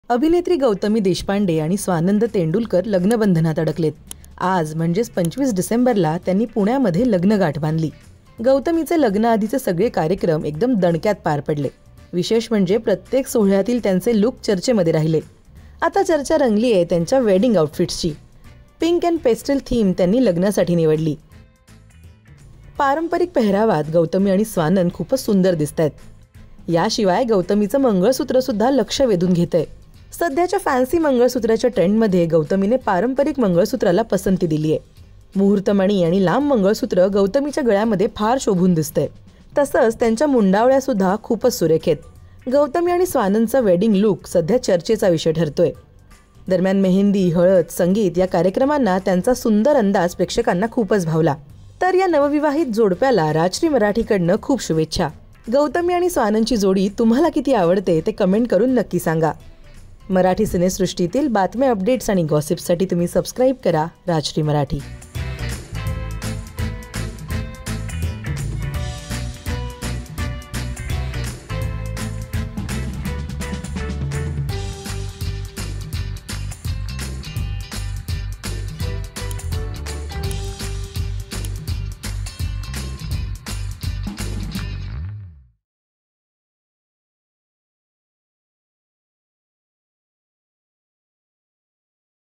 गौतमीची पारंपरिक मंगळसूत्राला पसंती | Traditional Mangalsutra | SwaG | Gautami - Swanand Wedding
अभिनेत्री गौतमी देशपांडे आणि स्वानंद तेंडुलकर लग्नबंधनात अडकलेत. गौतमीने सुंदर पारंपरिक मंगळसूत्राला पसंती दिली आहे. पहा त्यांचा सुंदर लूक.